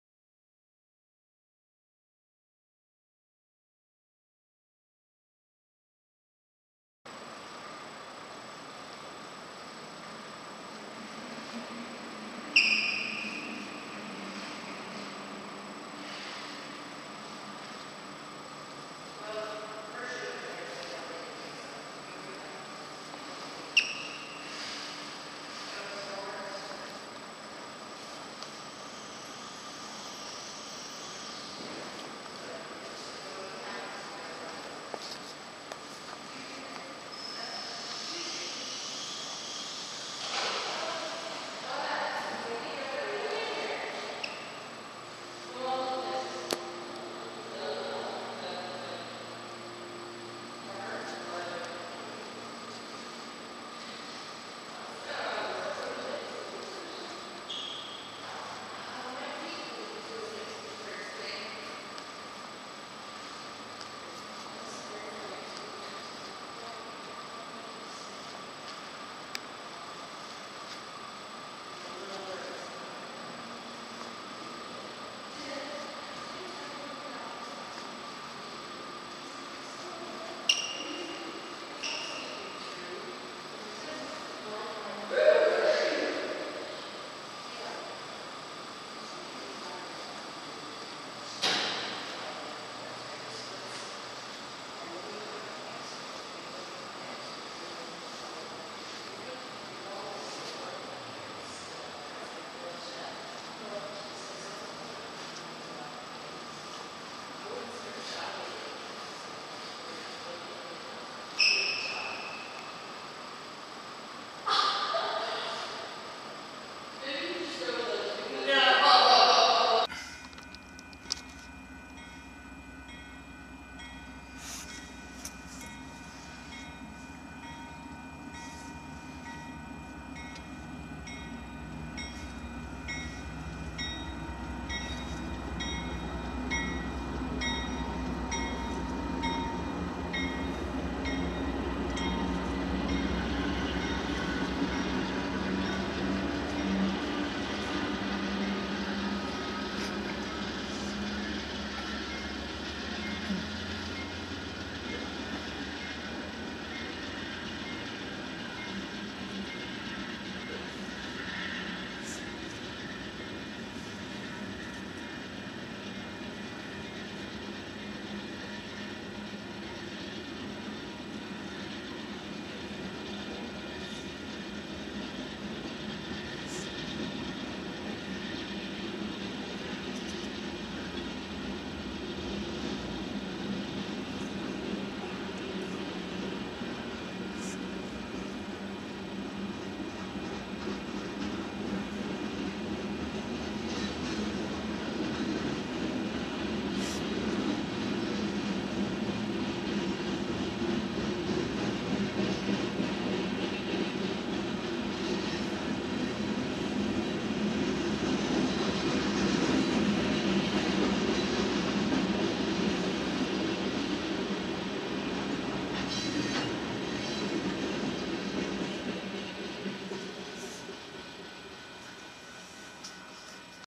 NB UPRR Hopper Car Train Moves Thru Austin TX
10/16